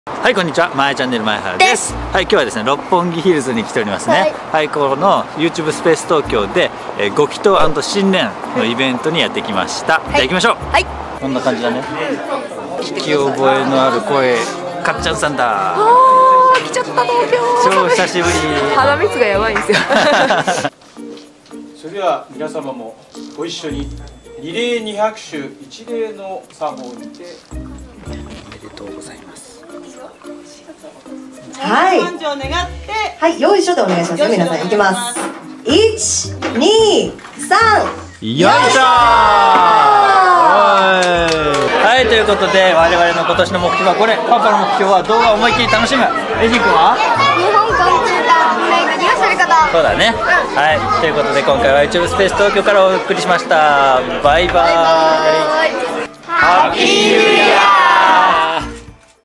人気祈願！ YouTube Space Tokyoのご祈祷&新年会イベントへ行ってきました！ | まえちゃんねる
2017年1月14日本日、YouTube Space Tokyoにて行われた「ご祈祷&新年会イベント」へ行ってきました！
神主さんがYouTube Space Tokyoにてチャンネル繁盛祈願を執り行っていただきました^^

今年はチャンネル繁盛しますように！頑張るぞ！